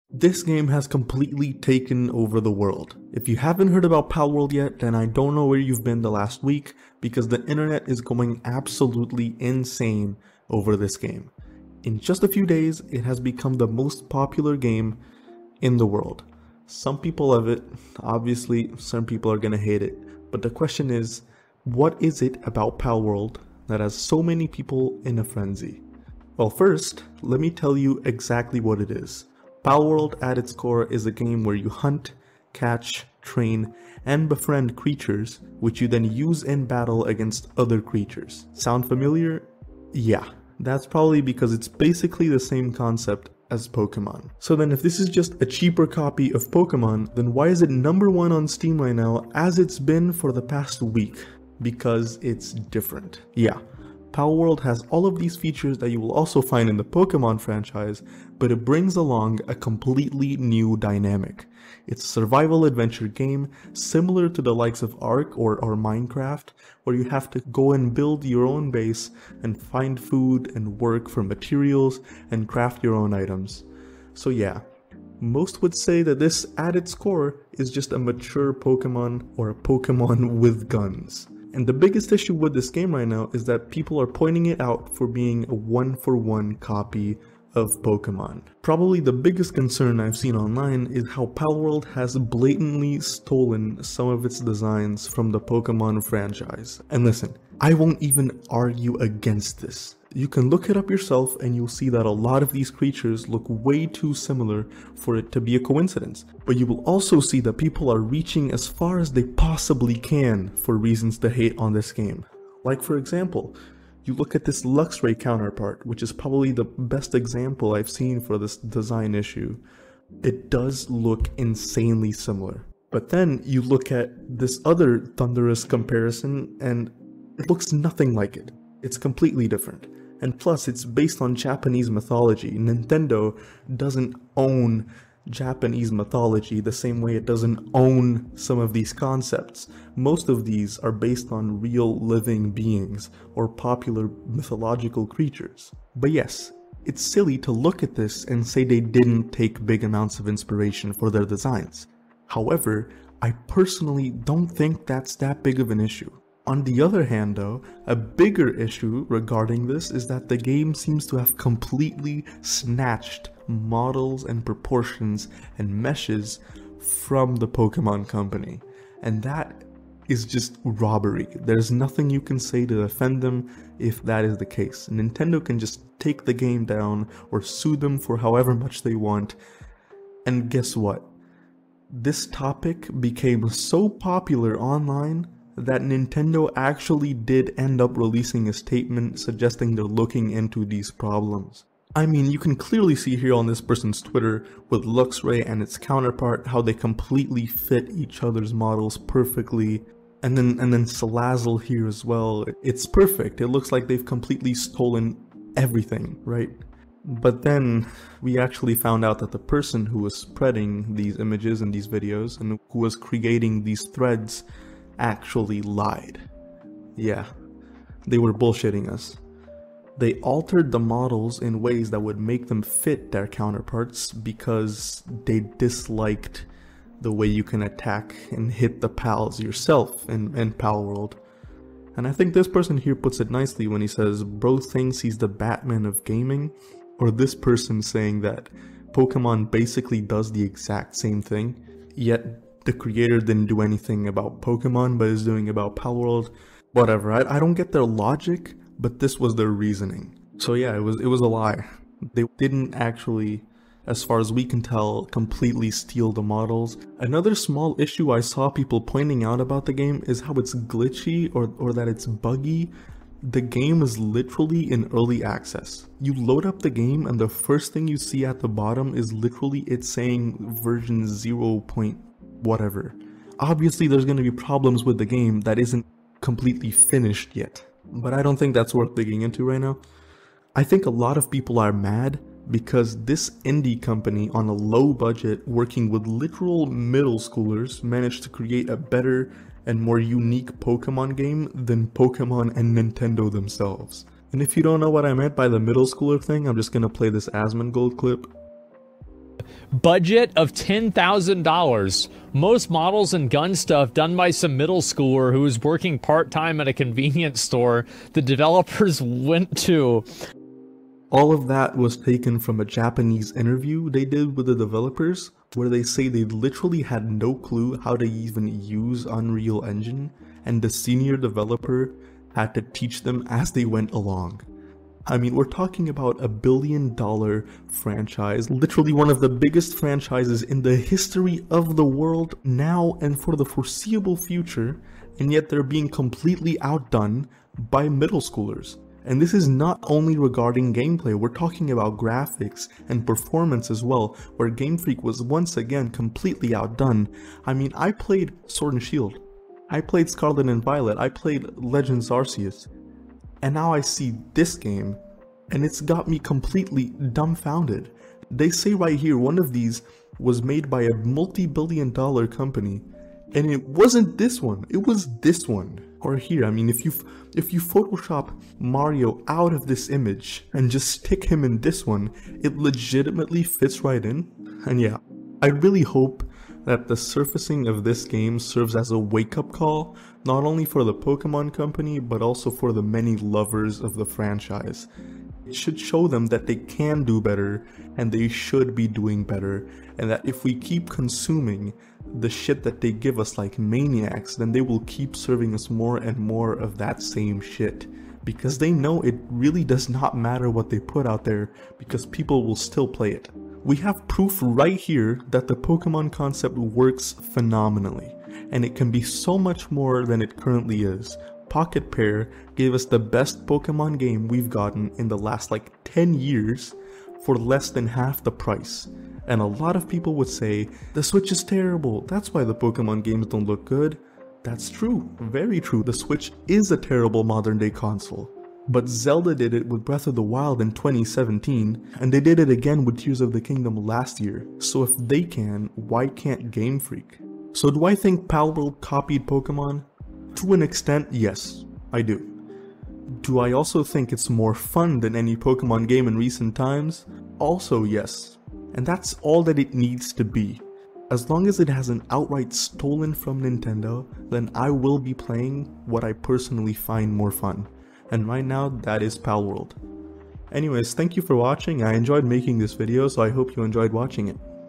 Palworld is its own game.
I've seen a lot of criticism towards palworld these last few days over its nature and its many resemblances to to Pokemon, I decided to talk a little bit about it and give my opinion on the matter.

00:00 - What is Palworld?
00:44 - Why is everyone talking about Palworld
01:28 - The biggest issue regarding Palworld
02:42 - Did the game steal models?
04:17 - The game is buggy
04:43 - Why are so many people mad?
05:46 - Game Freak needs to do better
06:15 - Conclusion